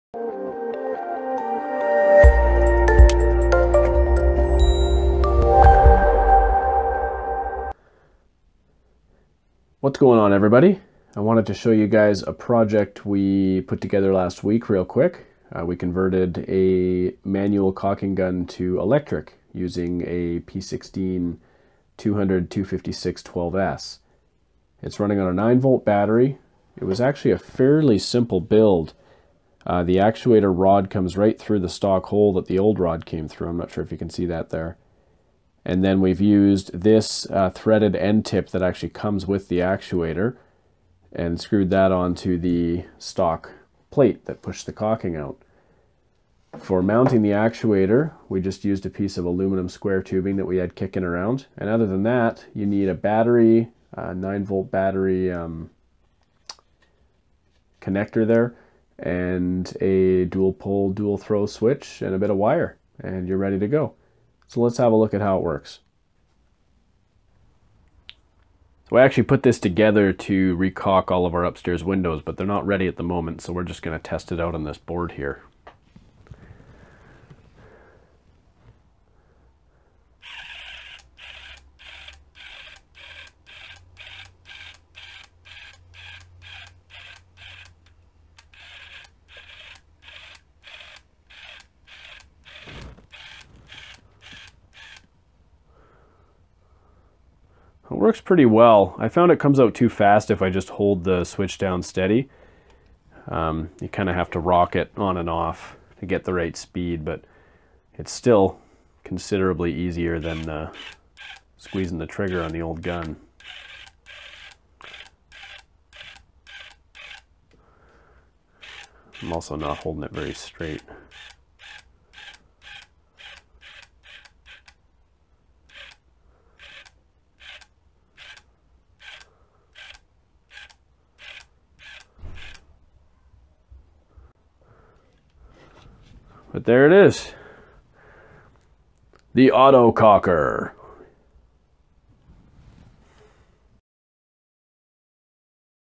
DIY Electric Caulking Gun
This week we built a DIY electric caulking gun. It works really well and uses a linear actuator and a 9V battery to push out the caulk.

The parts we used to build this project are listed below:

9V battery
9V battery connector
DPDT switch
Aluminum square tube for mounting

It can be difficult to push thick caulk through a manual caulking gun so we decided to see if we could build a cheap, diy caulking gun. It works fairly well but I think it would perform better on 12V. We are running a 12V actuator on 9V and reducing the voltage reduces power so it struggles a bit under a load.

If you've built a tool of some sort we'd love to see it. Leave a link below in the comments and show us what diy tools you've created.

Thanks for checking out this project and happy DIYing!